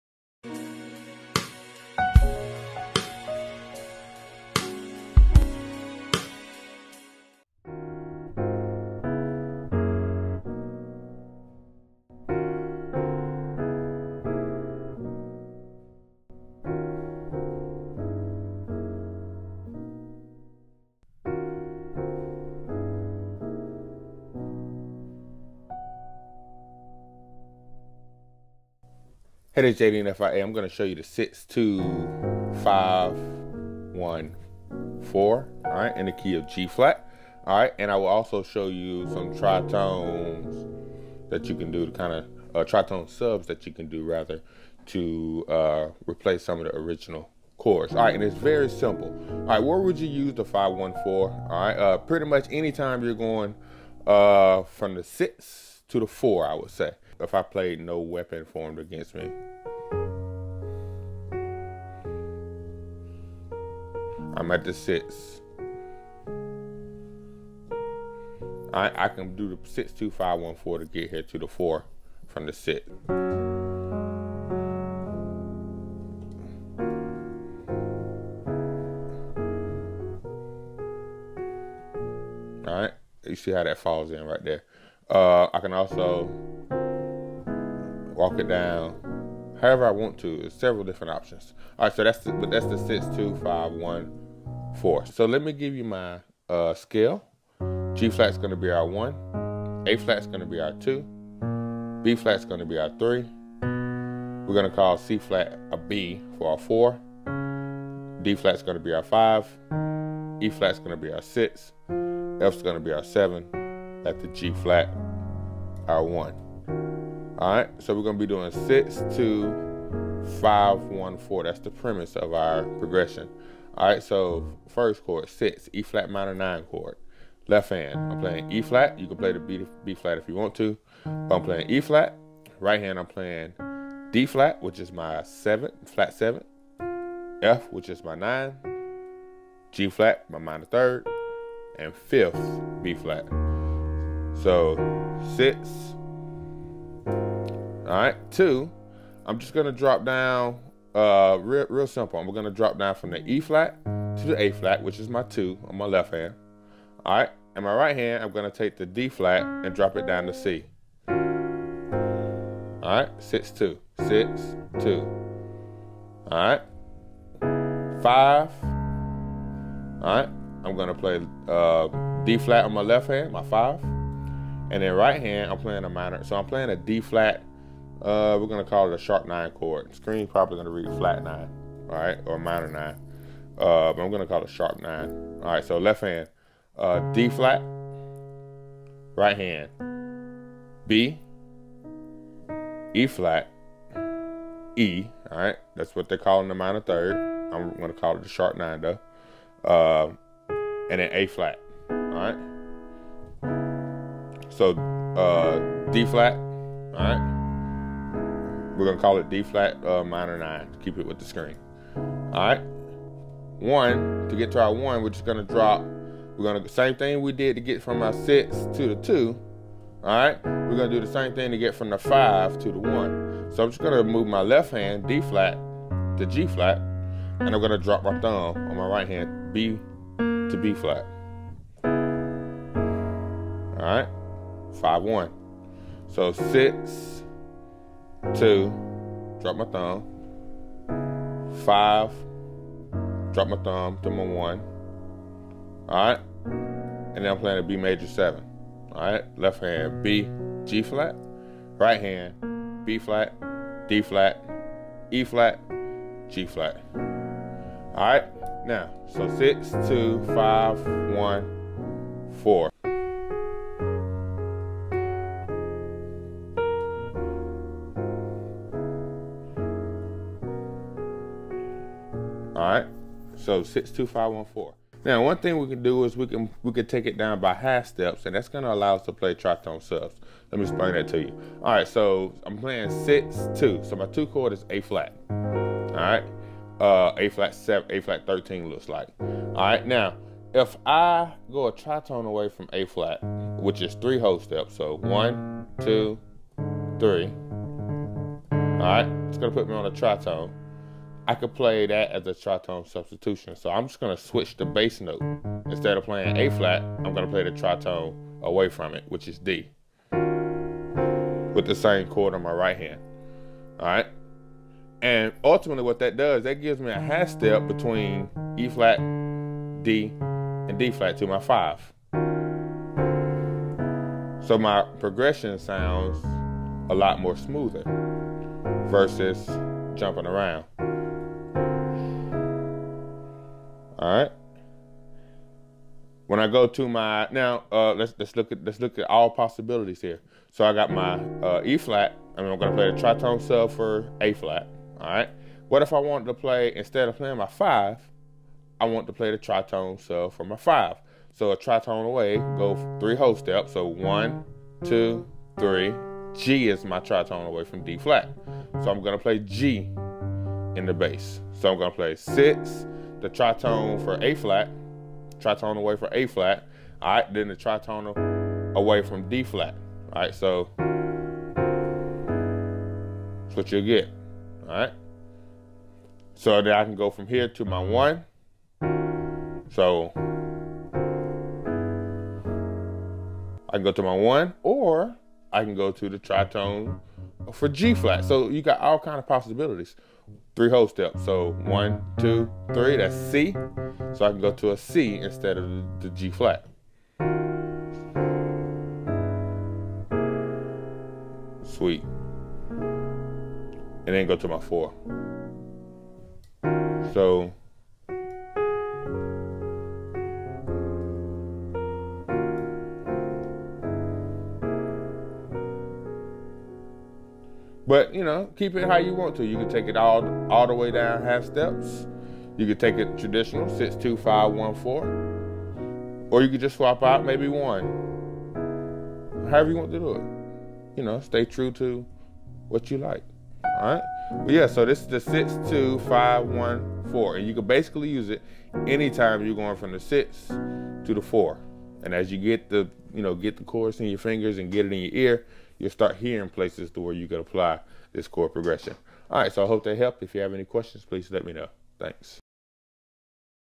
Gb F#: 6-2-5-1-4 Chord Progression with TriTone Substitutions
This video shows a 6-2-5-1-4 chord progression along with some tritone substitutions in the key of Gb/ F#.